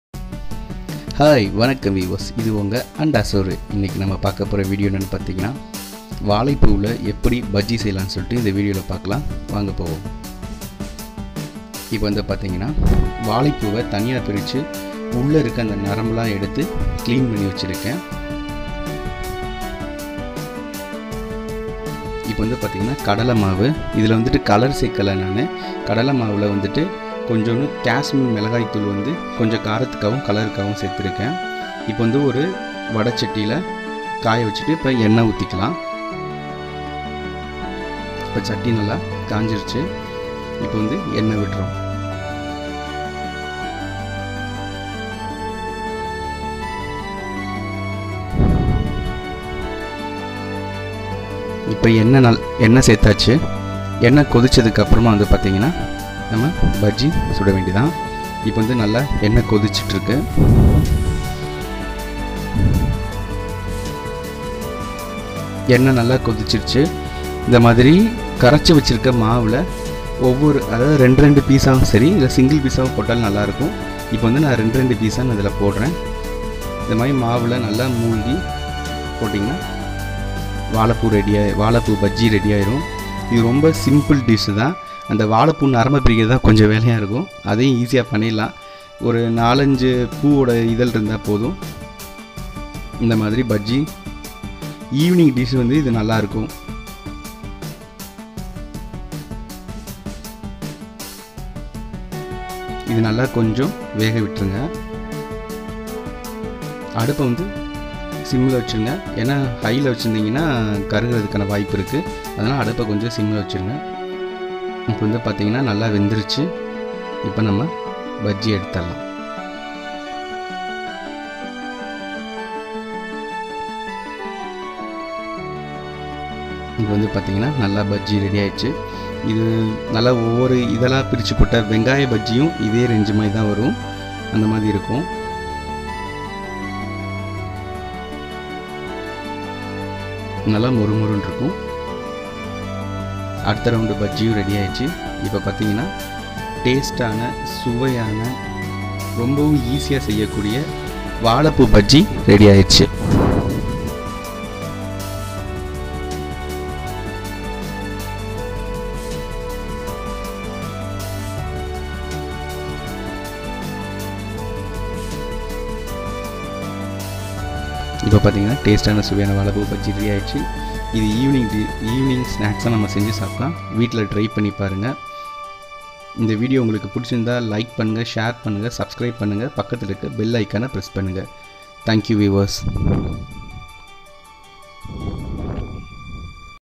fastamil banana flower bajji |வாழை பூ பஜ்ஜி | banana recipe
வாழை பூ1,கடலை மாவு,etc..
this video is how to cook banana flower bajji